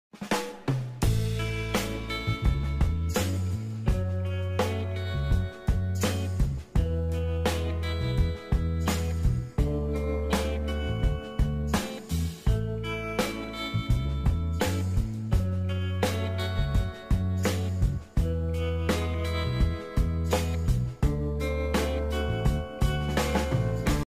Pure cotton handloom embroidered cross stitch dress material with price
Pure cotton handloom embroidered cross stitch top .pure cotton bottom and dupatta at best price